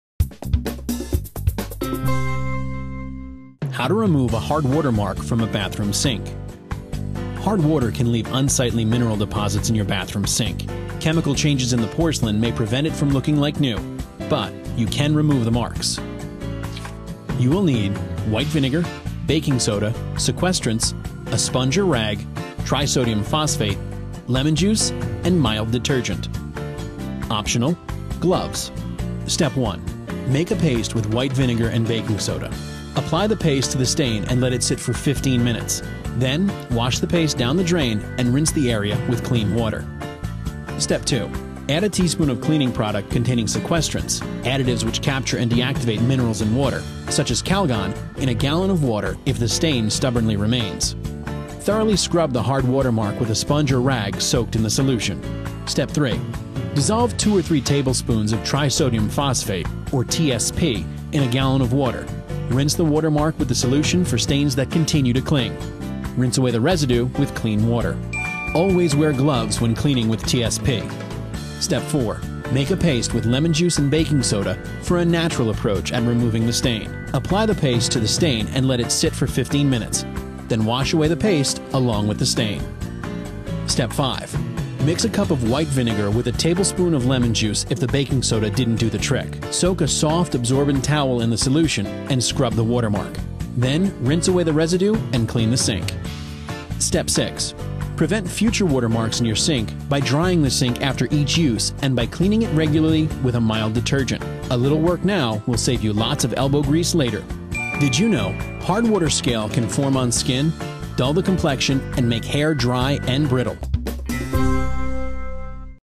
How to Remove a Hard Water Mark from a Bathroom Sink
Hard water can leave unsightly mineral deposits in your bathroom sink. Chemical changes in the porcelain may prevent it from looking like new, but you can remove the marks.

Step 1: Use vinegar and baking soda
Make a paste with white vinegar and baking soda. Apply the paste to the stain and let it sit for 15 minutes. Then wash the paste down the drain and rinse the area with clean water.

Step 2: Try sequestrants
Add a teaspoon of a cleaning product containing sequestrants, additives which capture and deactivate minerals in water, such as Calgon, in a gallon of water if the stain stubbornly remains. Thoroughly scrub the hard water mark with a sponge or rag soaked in the solution.

Step 3: Use TSP
Dissolve 2 to 3 tablespoons of trisodium phosphate, or TSP, in a gallon of water, rinse the water mark with the solution for stains that continue to cling. Rinse away residue with clean water.

Tip
Always wear gloves when cleaning with TSP.

Step 4: Use lemon juice
Make a paste with lemon juice and baking soda for a natural approach at removing the stain. Apply the paste to the stain and let it sit for 15 minutes. Then wash away the paste along with the stain.

Step 5: Use vinegar and lemon juice
Mix a cup of white vinegar with a tablespoon of lemon juice if the baking soda didn't do the trick. Soak a soft, absorbent towel in the solution and scrub the water mark. Then rinse away the residue and clean the sink.

Step 6: Prevent future marks
Prevent future water marks in your sink by drying the sink after each use and by cleaning it regularly with a mild detergent. A little work now will save you lots of elbow grease later.

Hard water scale can form on skin, dull the complexion, and make hair dry and brittle.